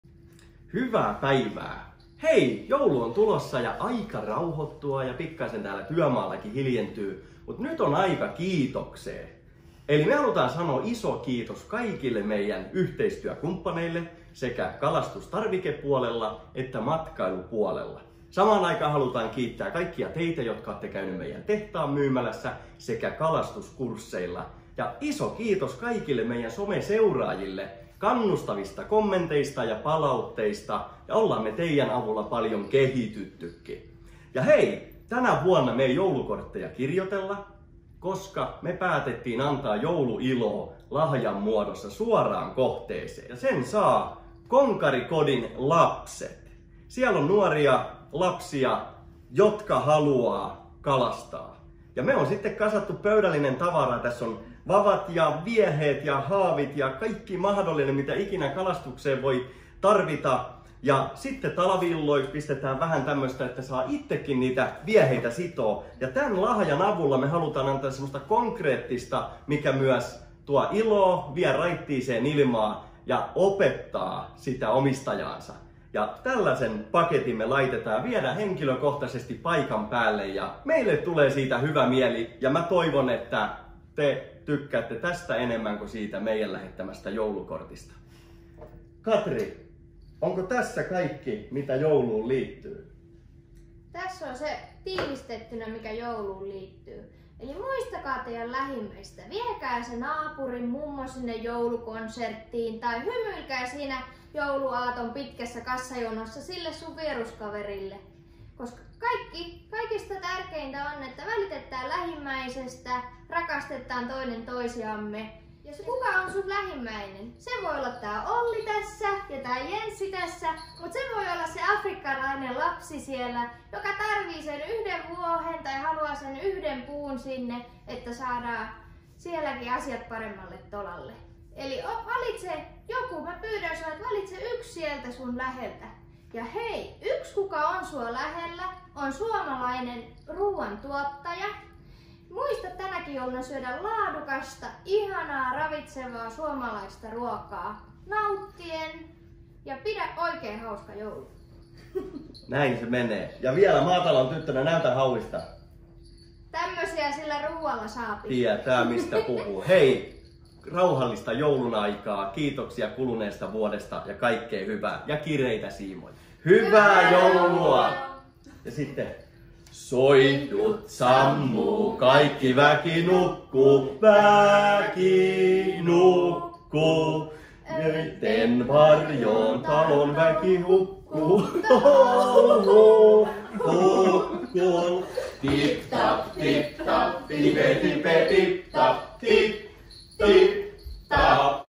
Leppoisaa ja rauhallista joulua!🎣
Eumerin väki kiittää kaikkia ystäviä ja yhteistyökumppaneita kuluneesta vuodesta.❤️Postikorttien sijaan annamme joululahjat suoraan Konkari-kodin lapsille.